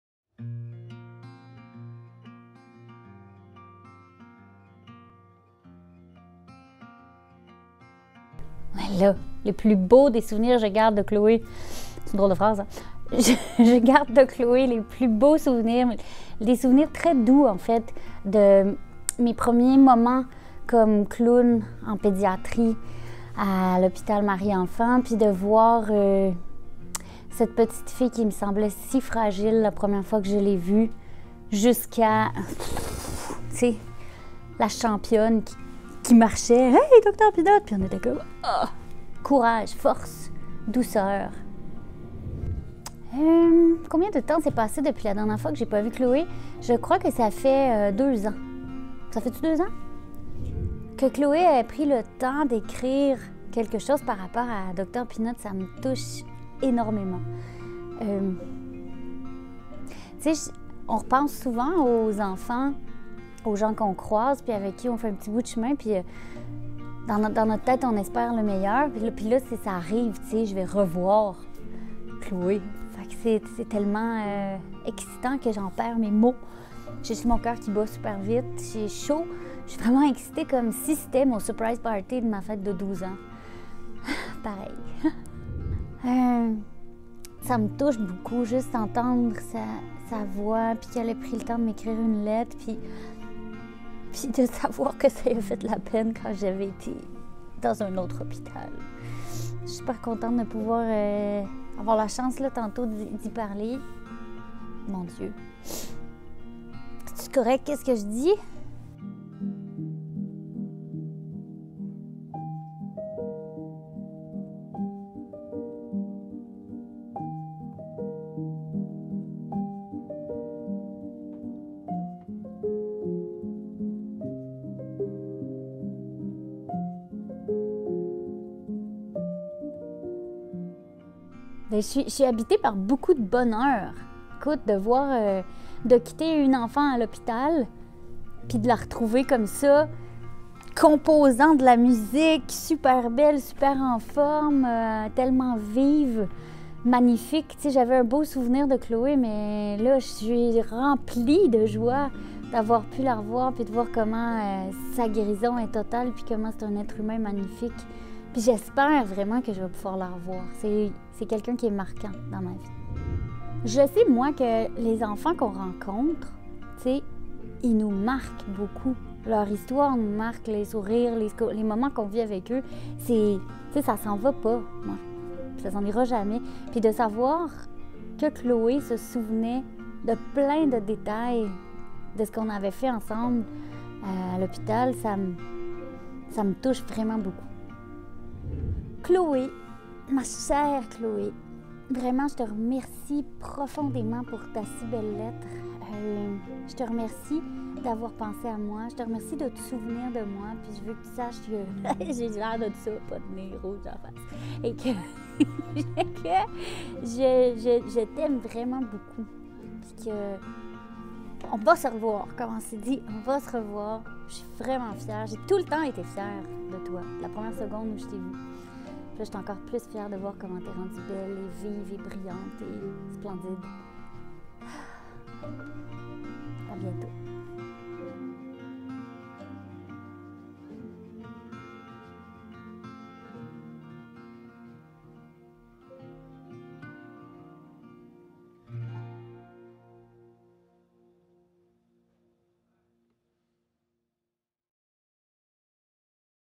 Les confidences de Geneviève
Dans cette vidéo exclusive, Geneviève nous partage les émotions qui l’habitent alors qu’elle s’apprête à retrouver Chloé, l’une des premières enfants qu’elle a visitées sur le terrain, en tant que Dre Clown. Elle nous confie également comment elle a vécu cette précieuse retrouvaille avec son amie.